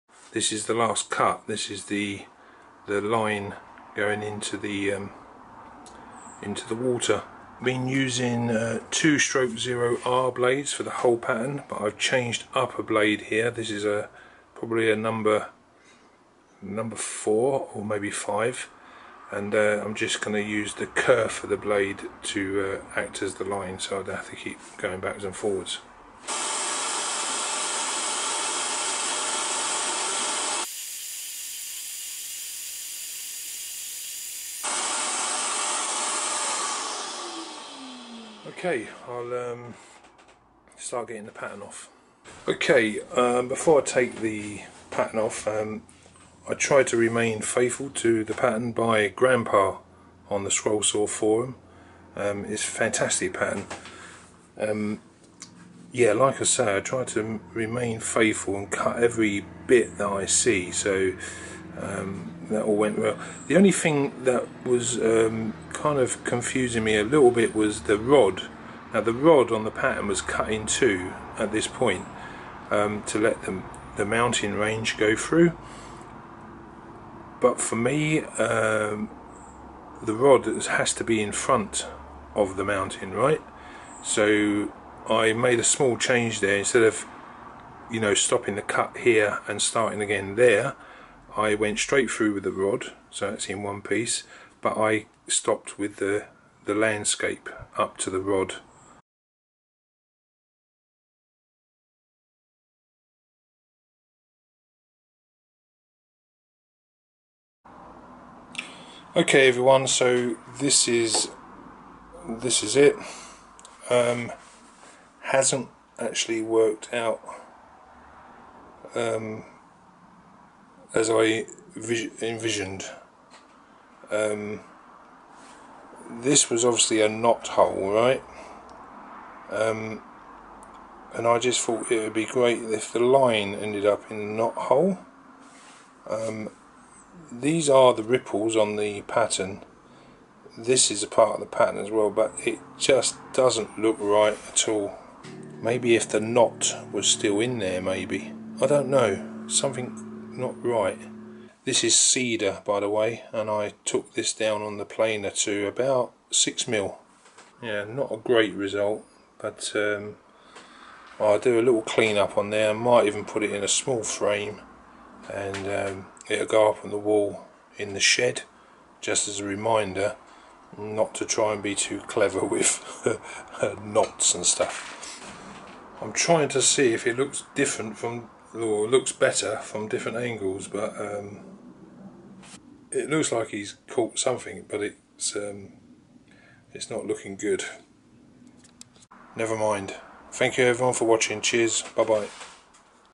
Fishing scrollsaw pattern
The pattern I found on scroll saw village by Grampa.

Wood is Cedar of Lebanon.
I used a Olsen 2/0 blade for the pattern changing to a No5 for the fishing line.

It is a great pattern but I managed to mess it up. 😃